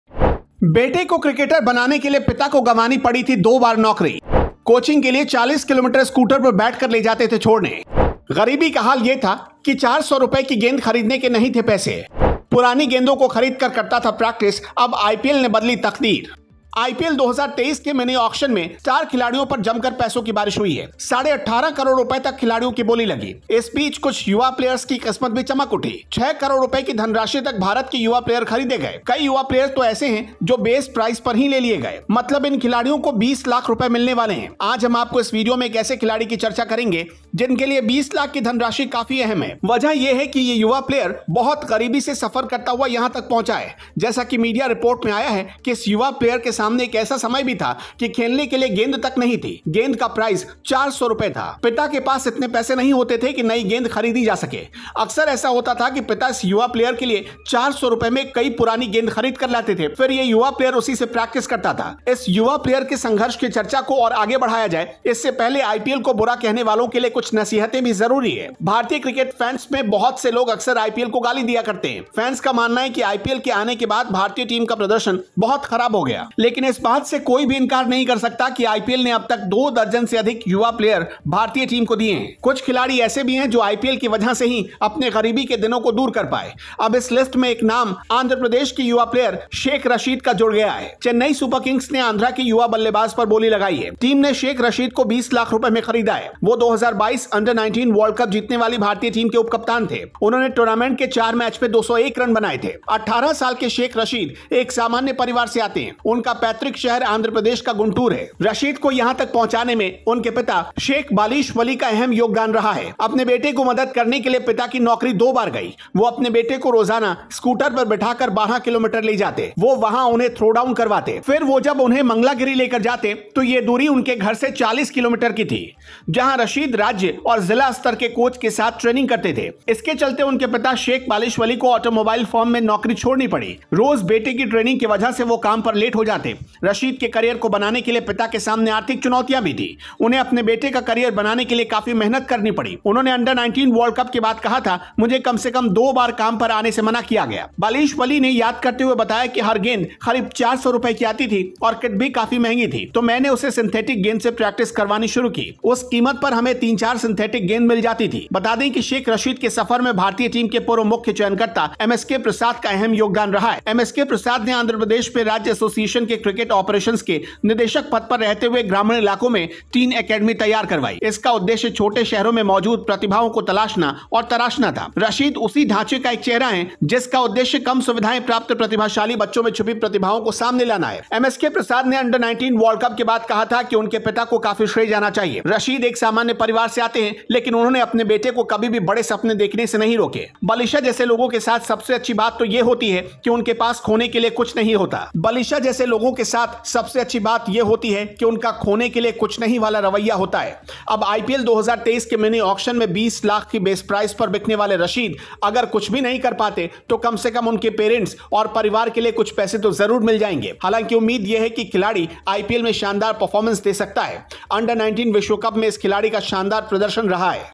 जिस क्रिकेटर के पास बॉल खरीदने के नहीं थे पैसे, एक रात में बन गया करोड़पति
इतना गरीब था यह क्रिकेटर बॉल खरीदने के नहीं थे पैसे, पुरानी गेंदों से किया प्रैक्टिस, बेटे को क्रिकेटर बनाने के चक्कर में पिता की दो बार चली गई नौकरी, आज हो गया है करोड़पति